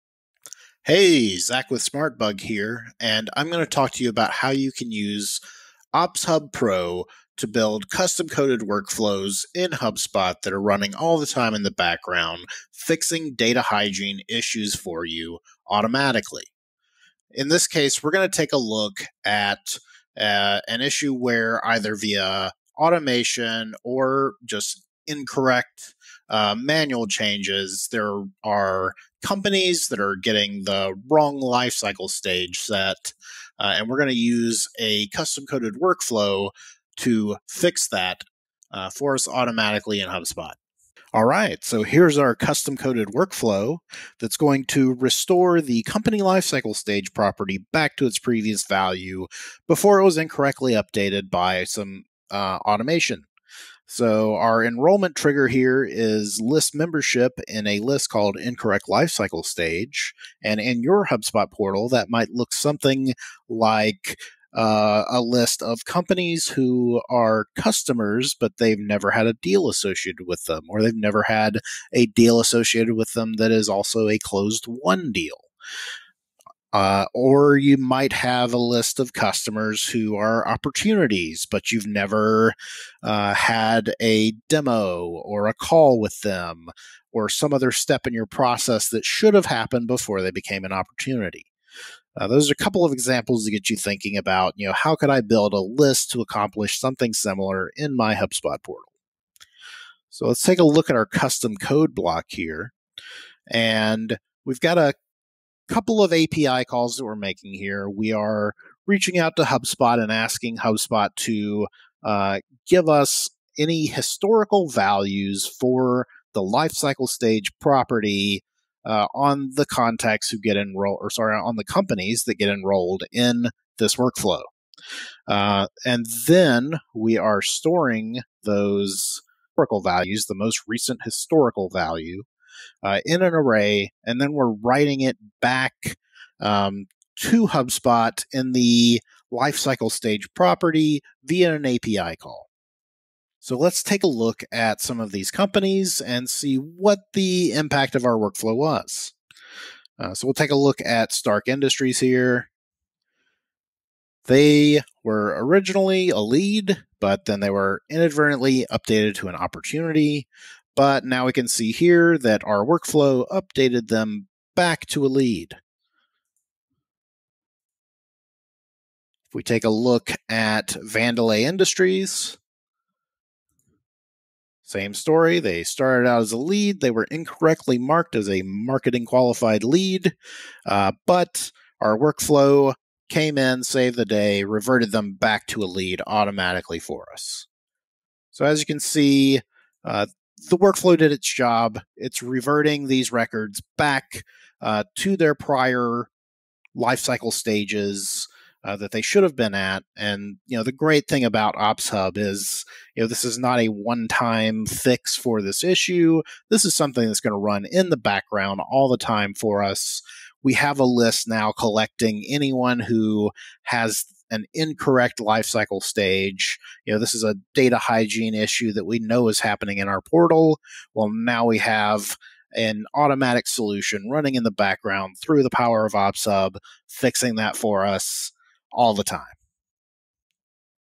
Improve HubSpot Data Hygiene: Clean and Boost Your CRM Performance With This Workflow
Are you struggling with your HubSpot data hygiene? Not enough time or resources to keep your data clean and organized? Discover the secrets to maintaining impeccable HubSpot data hygiene in this custom tutorial. Learn how to efficiently manage your contacts, eliminate duplicates, and streamline your workflows – all while boosting your CRM performance. Don't let messy data hold you back! Dive into our step-by-step guide and unlock the full potential of your HubSpot CRM today with this tip from Zach. Master your HubSpot data hygiene!

This video is an example of a custom-coded workflow that allows you to add automated guardrails reverting incorrect changes to company lifecycle stages, whether they're done via workflow automation or through manual changes to the records. All in an effort to improve the hygiene of your data in HubSpot.

Database Cleanup: How to Clean Bad Data and Duplicates in Your CRM ➡️

00:00 Introduction
00:16 Overview of the problem
00:41 Demo of solution